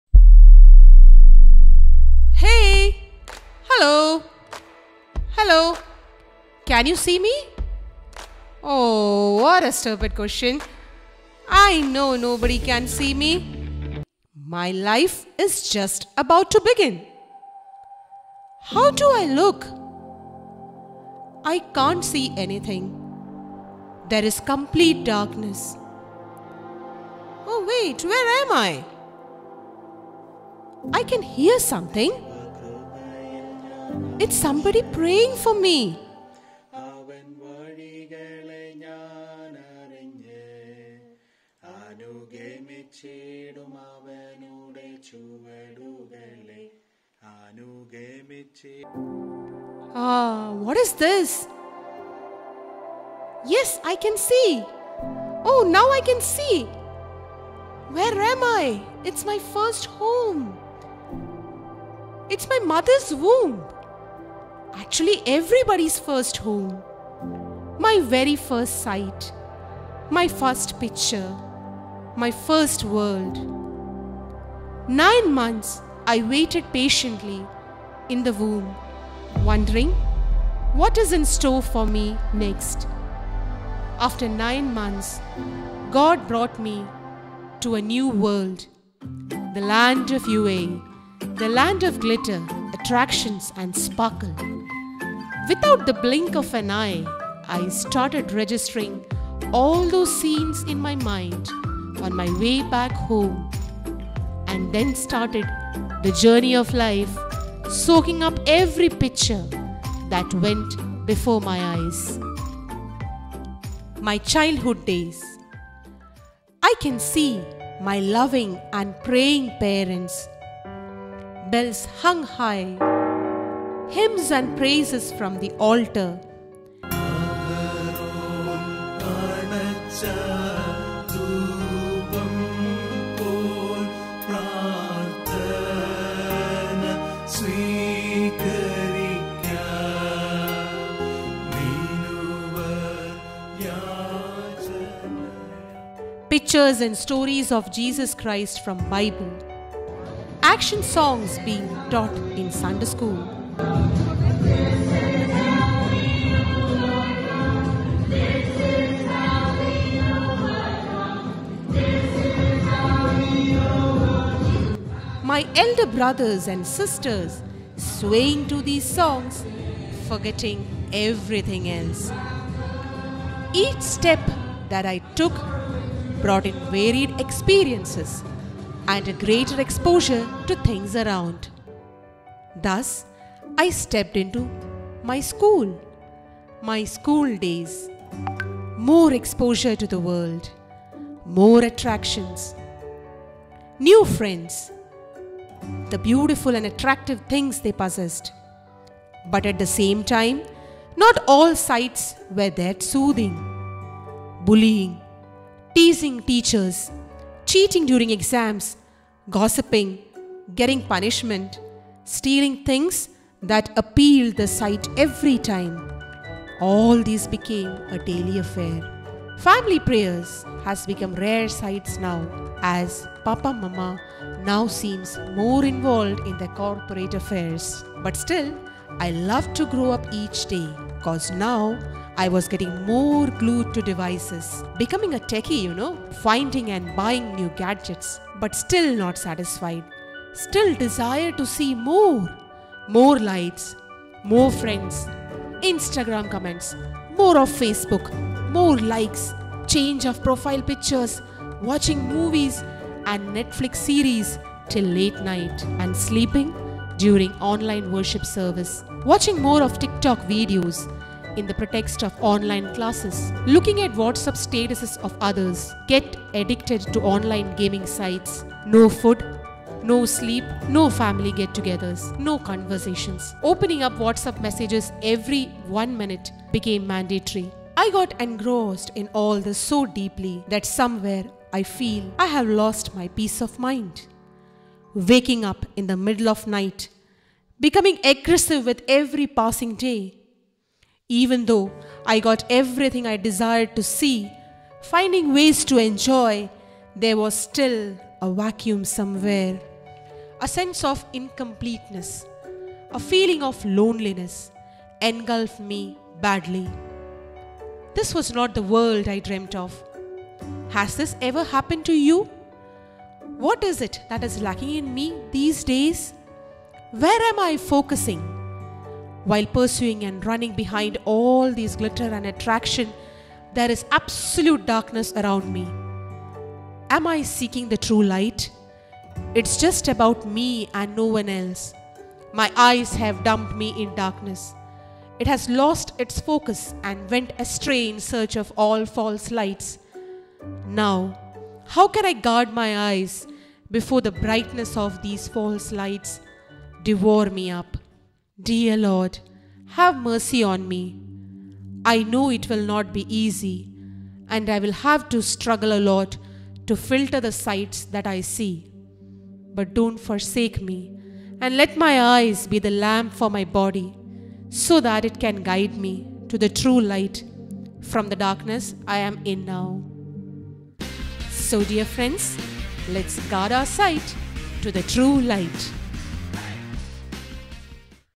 | SKIT | GUARDING YOUR SIGHT | VBS 2021 DUBAI | SEE JESUS SEEK LIFE | GUARD SIGHT TO SEE TRUE LIGHT|
This video was made for the VBS of Dubai Mar Thoma Church.
The theme for the VBS was GUARDIANS and the Day's Theme on which this video was shown was ' GUARD YOUR SIGHT '
What we see matters and so to guard ourselves we need  to guard our sight. Always be watchful about the enemy whose always on the look out to devour us.